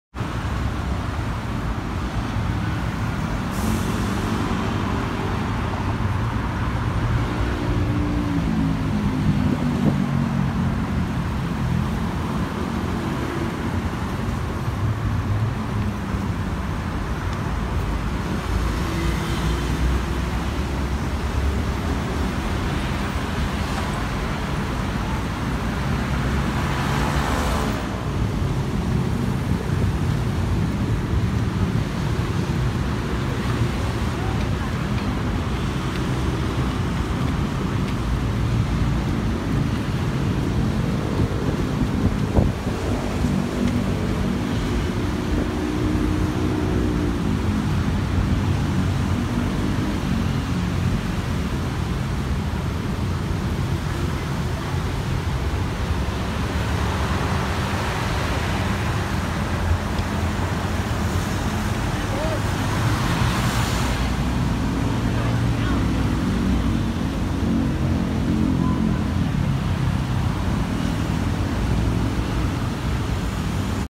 This Bridge Needs A Cable Tidy
This bridge down the road from the Huai Khwang MRT station in Bangkok looks like it needs a cable tidy to clean up the mess that pedestrians have to walk past.